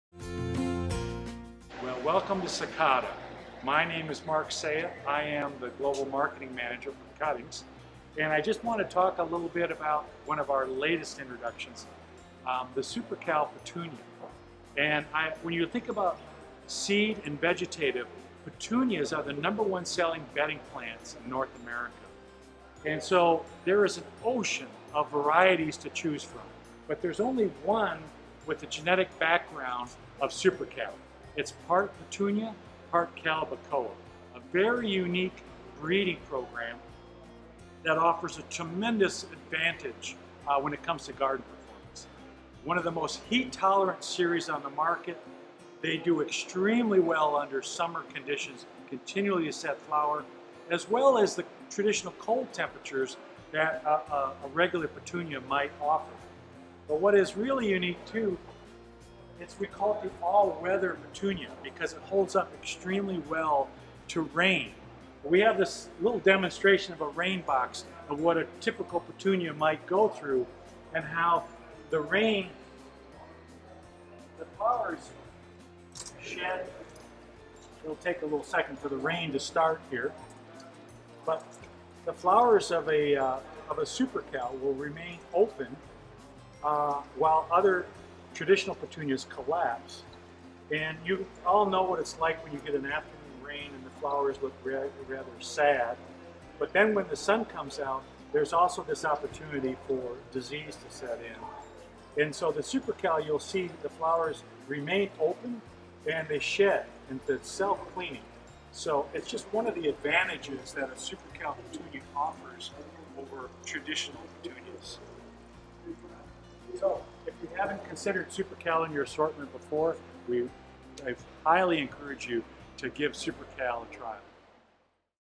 CAST 2022: SuperCal Petunia
At the 2022 California Spring Trials, Mark Seguin of Sakata Seed America conducts a live experiment with the SuperCal petunia to see whether the blooms hold up to the simulated rain.